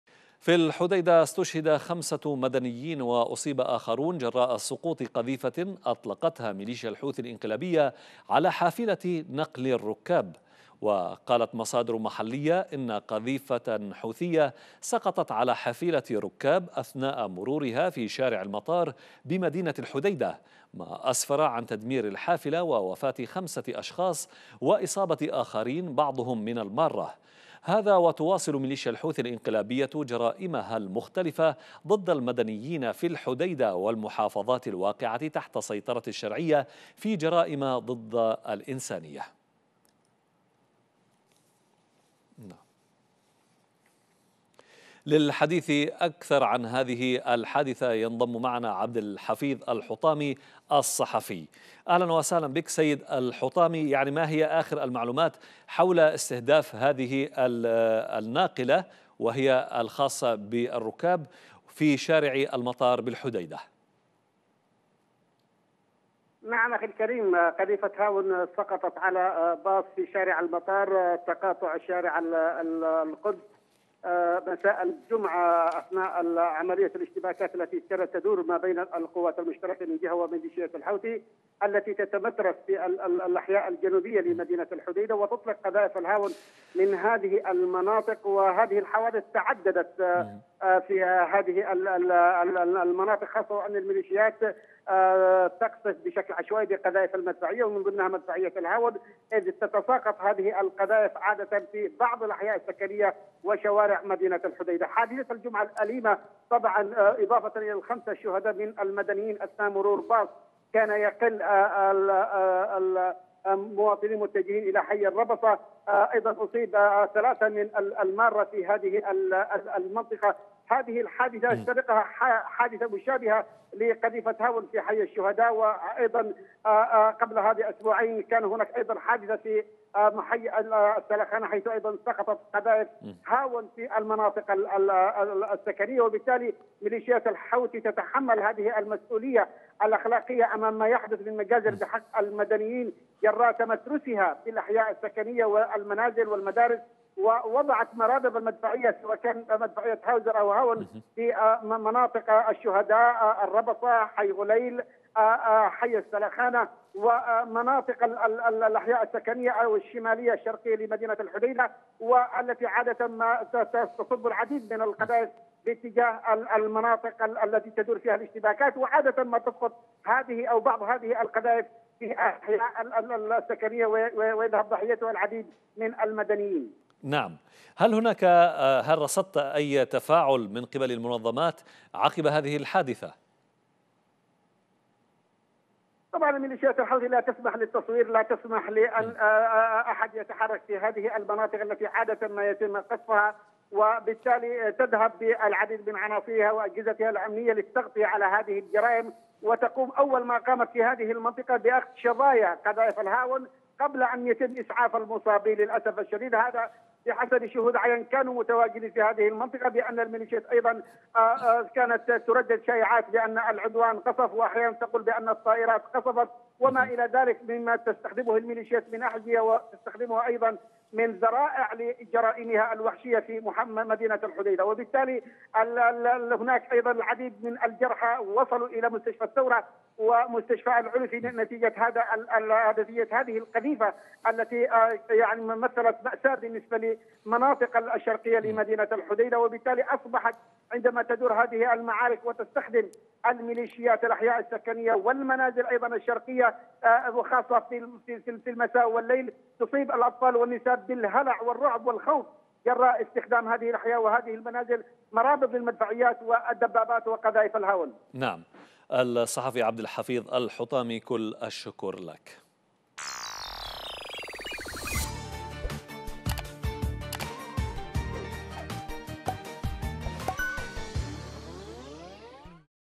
استشهاد 5 مدنيين بقذيفة حوثية استهدفت حافلة ركاب في الحديدة
|  تفاصيل اكثر مع الصحفي عبدالحفيظ الحطامي
قناة فضائية يمنية مستقلة تبث على مدار النايلسات بالترددات التالية :
HD     11257/H/27500
SD     11391/V/27500

موقعنا الإلكتروني والبث الحي : yemenshabab.net